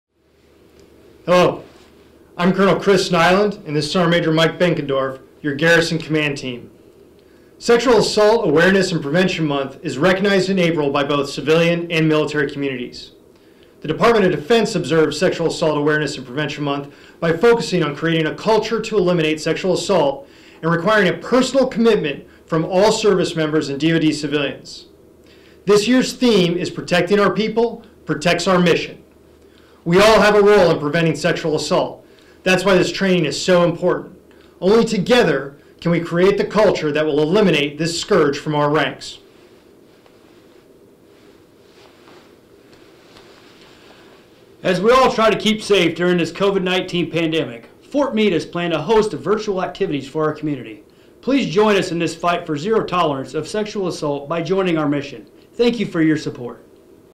SAAPM 2021
April is Sexual Assault and Abuse Prevention Month.  Here's a message from Fort Meade Garrison Commander, Colonel Chris Nyland and Command Sergeant Major Michael Behnkendorf.